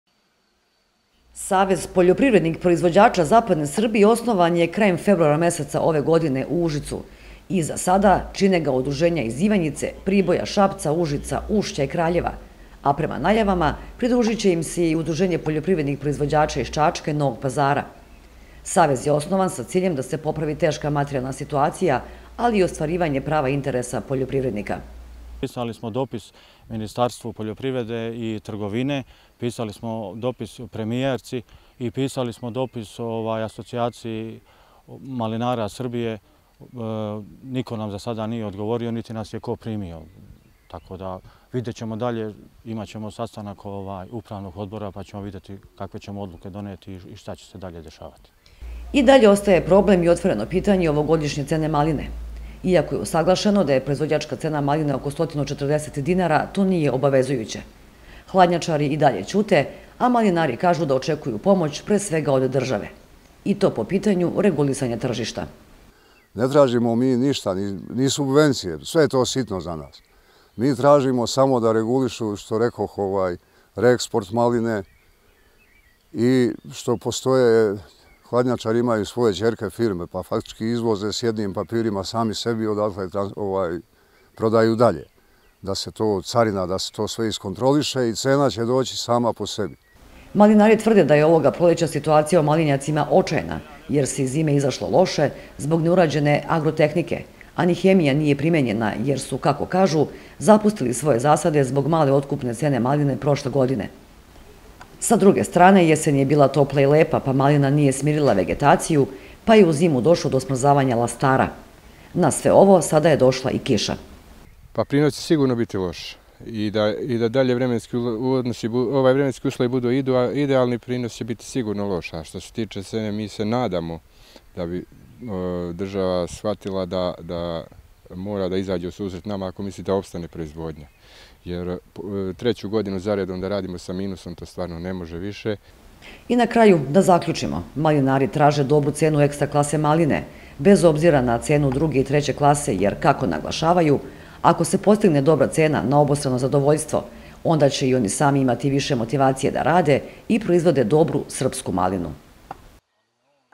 26.05.2019. ŠEST UDRUŽENJA MALINARA IZ ZAPADNE SRBIJE FORMIRALO SAVEZ
Sa ciljem da se popravi teška materijalna situacija, ali i ostvarivanje prava interesa poljoprivrednika, šest udruženja iz zapadne Srbije formiralo je savez. U toku su pregovori sa predstavnicima Vlade republike Srbije i ministarstvima poljoprivrede i trgovine da se sedne za isti sto i pokrenu pitanja zaštite srpske maline, nekontrolisanog uvoza i realne cene maline za ovu godinu.

STUDIO ČAČAK

Adresa: Skadarska 7/3, 32000 Čačak

STUDIO UŽICE